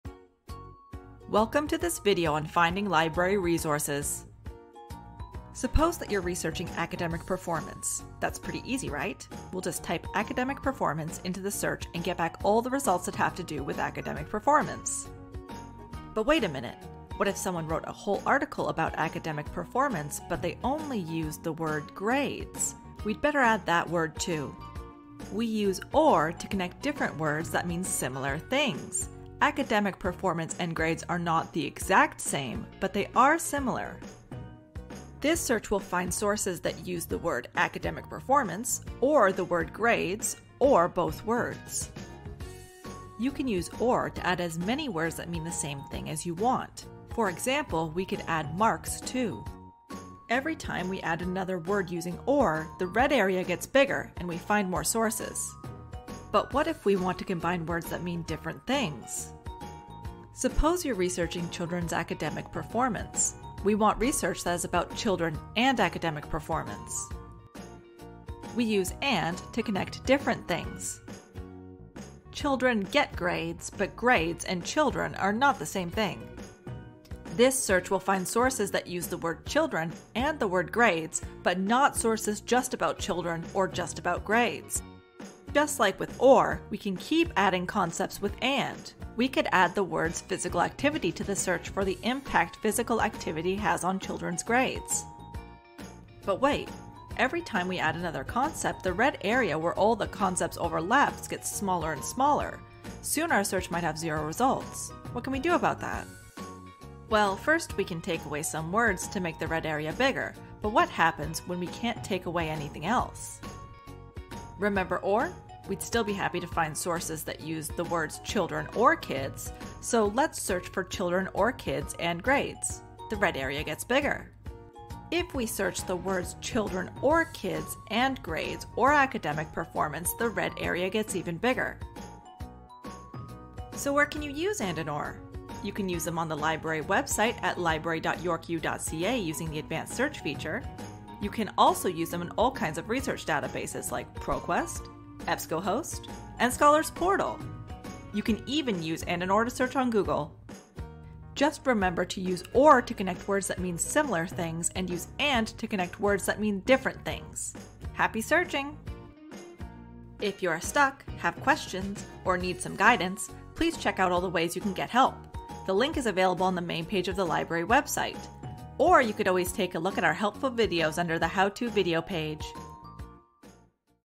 Searching with AND & OR
Learn how to do research using AND and OR.